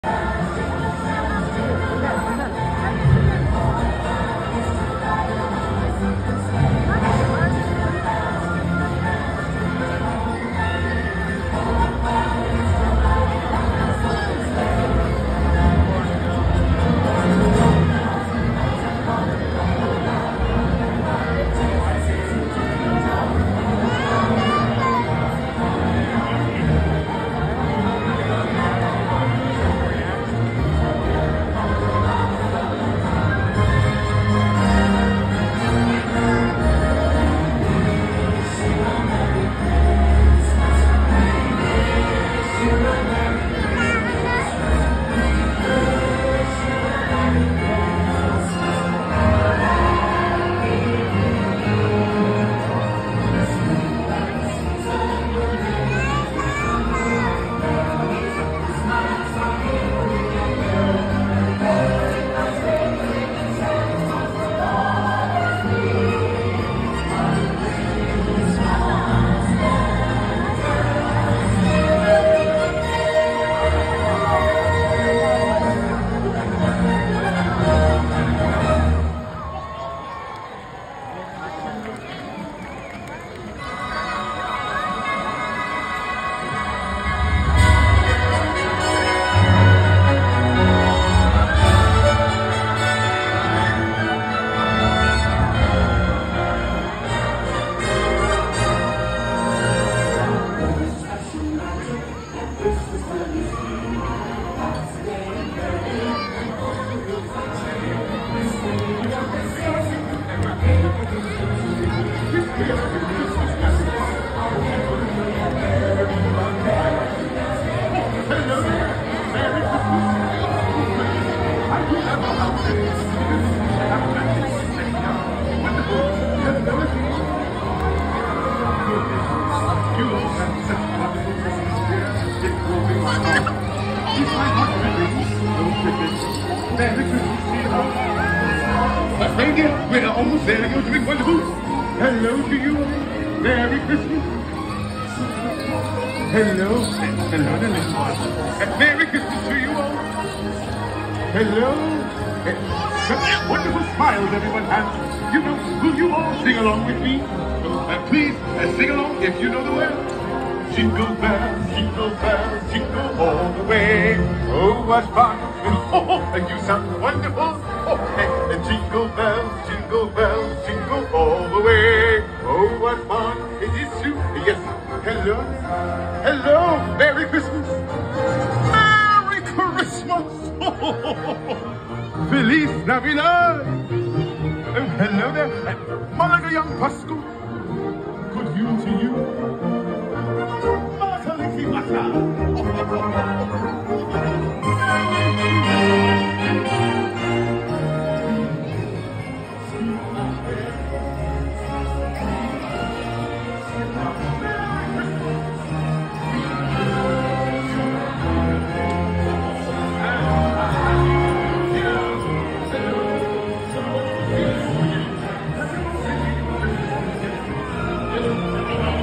The Final Part of A Christmas Fantasy Parade at Disneyland with Santa Clause and His Reindeer
Hey Mouseketeers! I hope you enjoy this recording of A Christmas Fantasy Parade at Disneyland Park in Anaheim, California. This is the sixth and final part of the parade which includes Santa Clause and his Reindeer! Happy Holidays!

Video quality: SD and HD

See ya real soon!